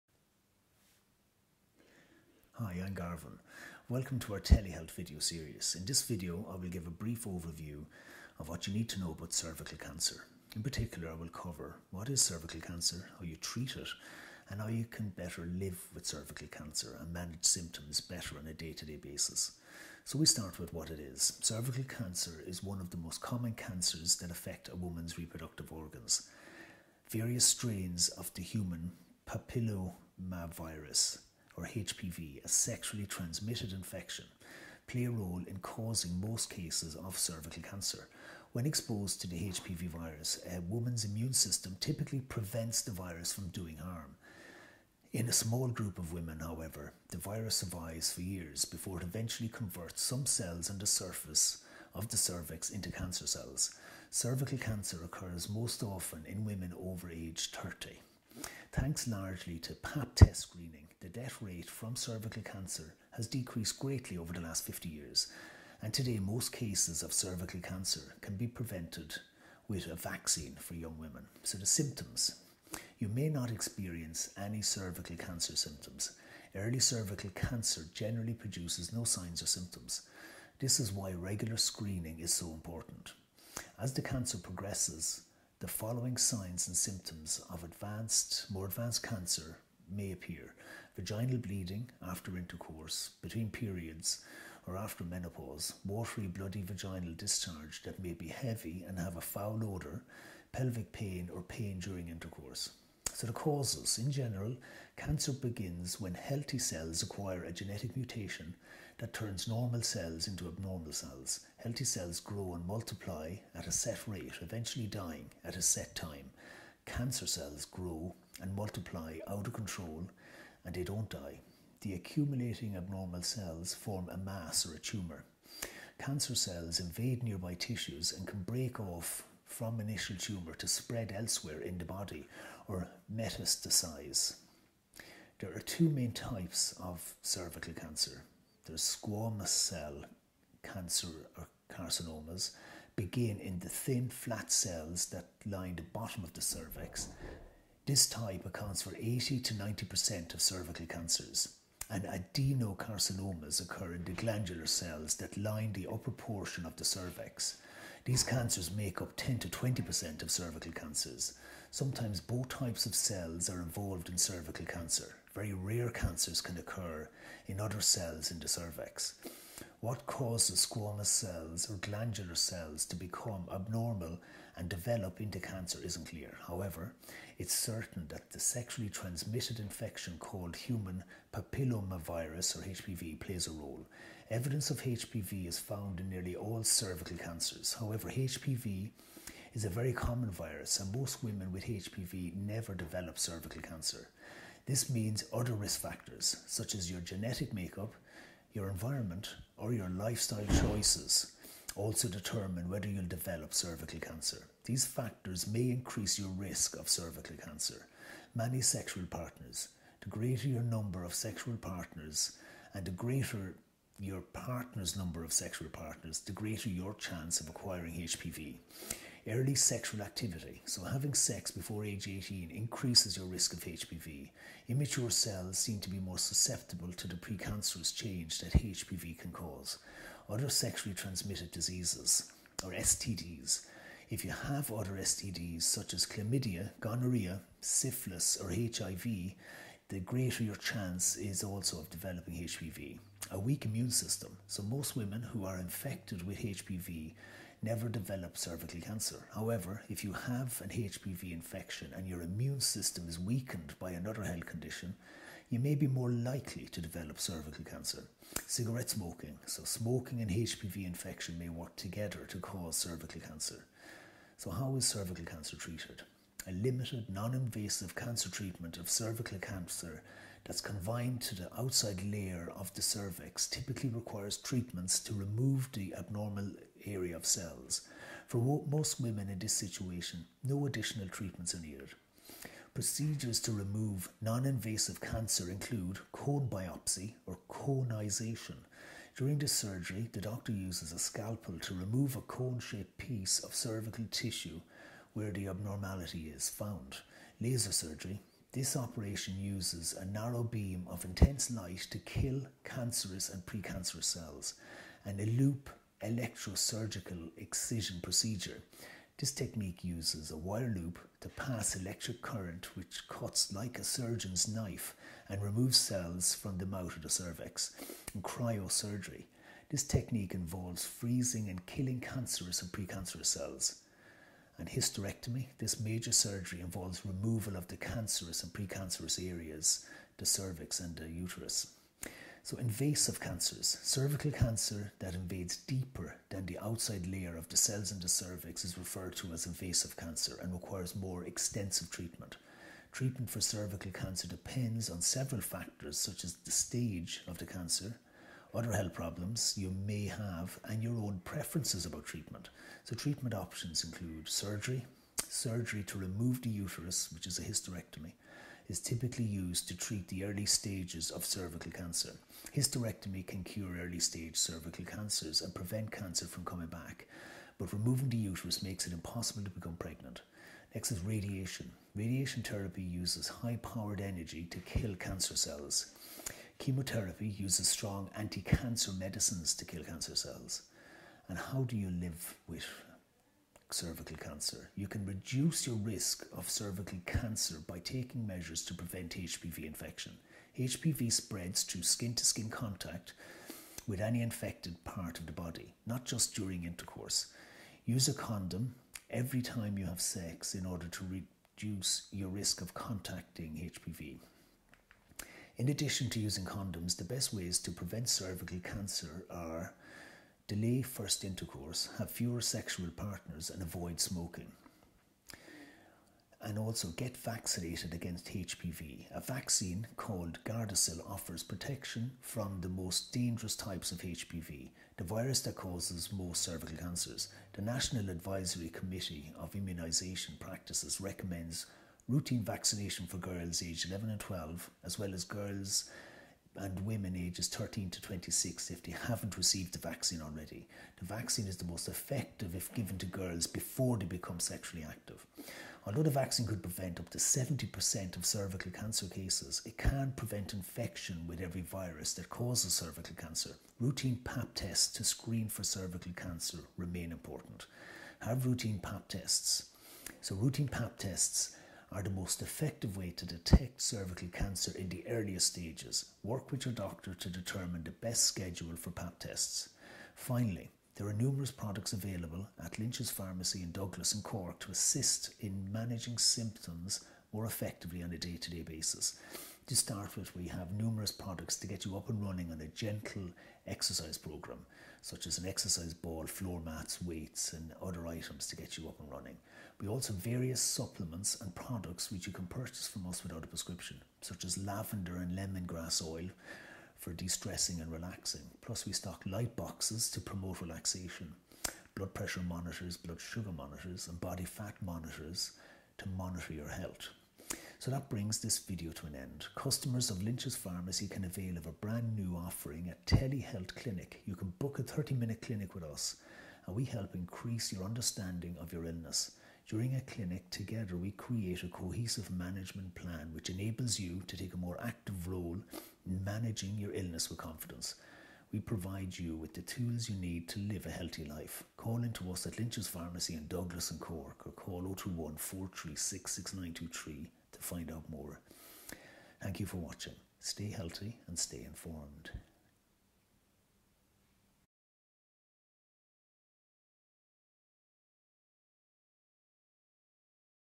Cervical Cancer - What you need to know.
Cervical cancer is one of the most common cancers that affect a woman's reproductive organs. Various strains of the human papillomavirus (HPV), a sexually transmitted infection, play a role in causing most cases of cervical cancer.
When exposed to HPV, a woman's immune system typically prevents the virus from doing harm. In a small group of women, however, the virus survives for years before it eventually converts some cells on the surface of the cervix into cancer cells. Cervical cancer occurs most often in women over age 30.
Thanks largely to Pap test screening, the death rate from cervical cancer has decreased greatly over the last 50 years.